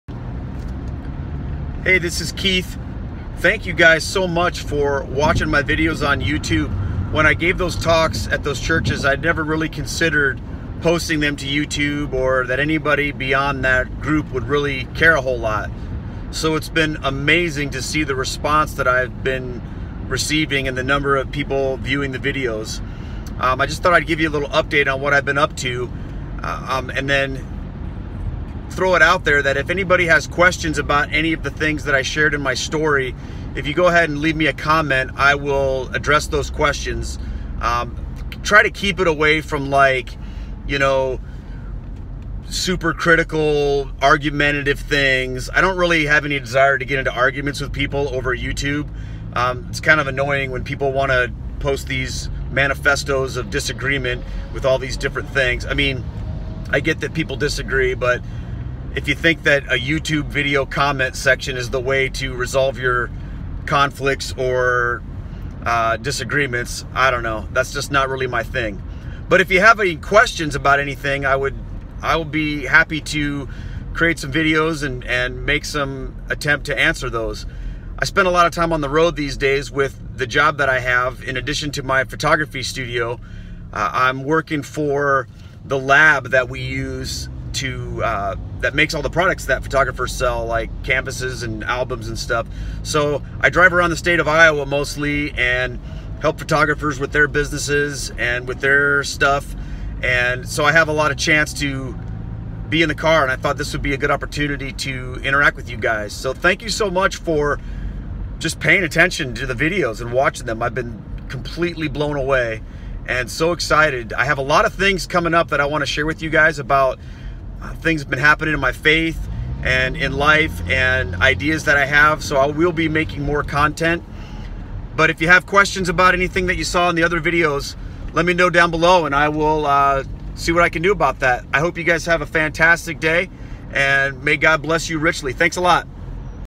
What’s Keith doing?
A quick update and invitation to ask questions about my conversion to Roman Catholicism.